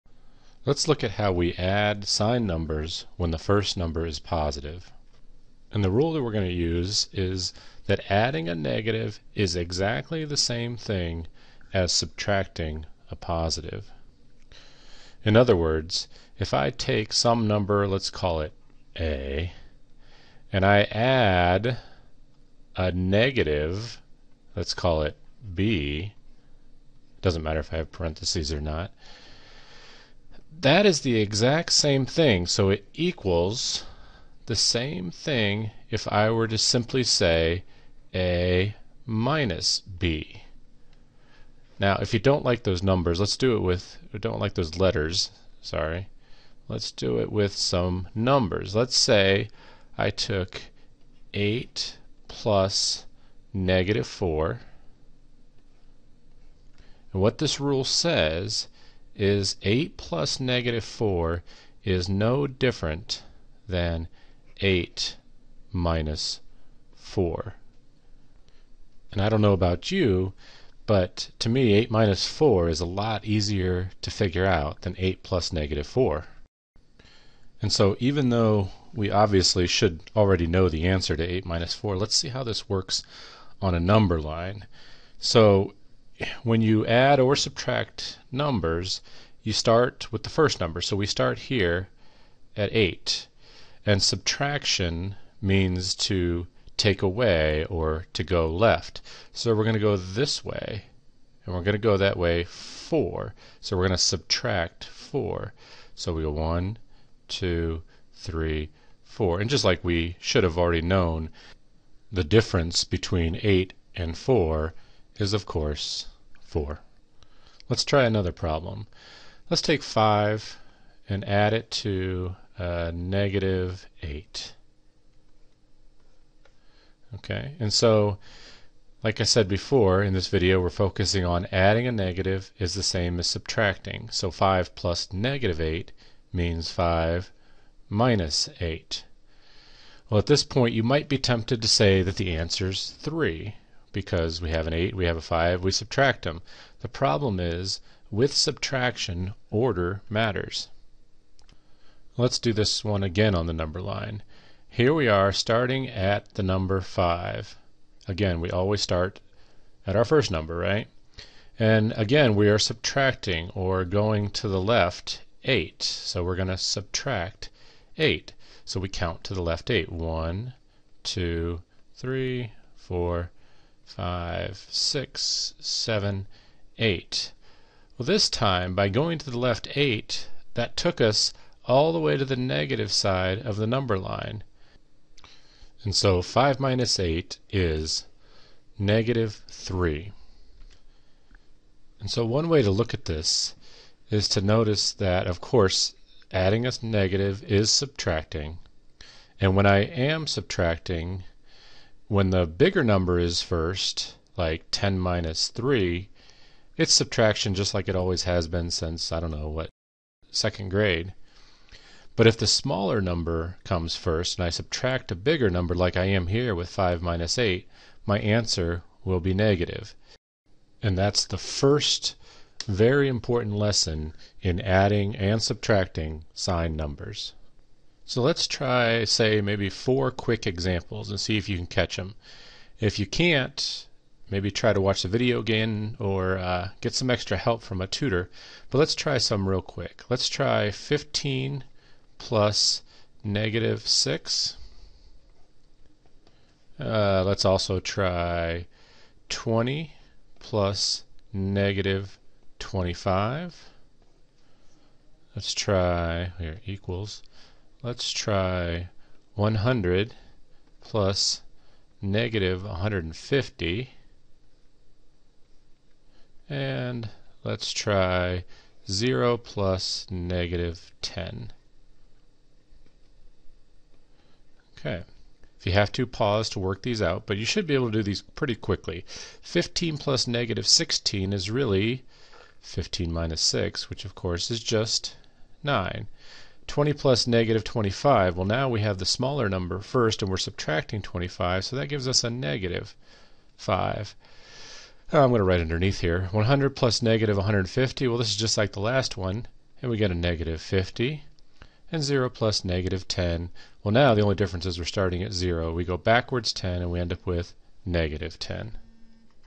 Adding a negative to a positive
Adding signed numbers where the first number is positive and the second is negative.